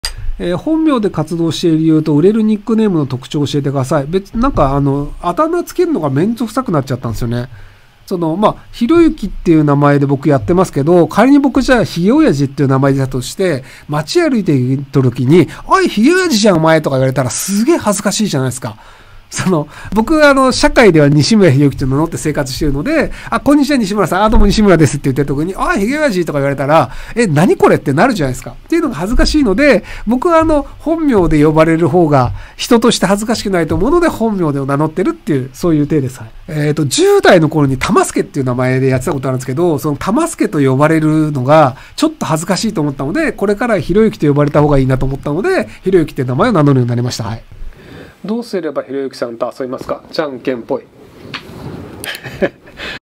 【ひろゆき】※何で本名で活動してるの？※ひろゆきが”ひろゆき”になる前のハンドルネームは◯◯すけ！？
風助でも宮助でもなくて"多摩助"
あめぞう時代のHNです。

当チャンネルは動画管理元ガジェット通信様の許可の元運営しております。

【元動画】
【ひろゆき】鴨は逆転を信じ騙される。Bière du Corbeau を呑みながら。2021/03/17 M00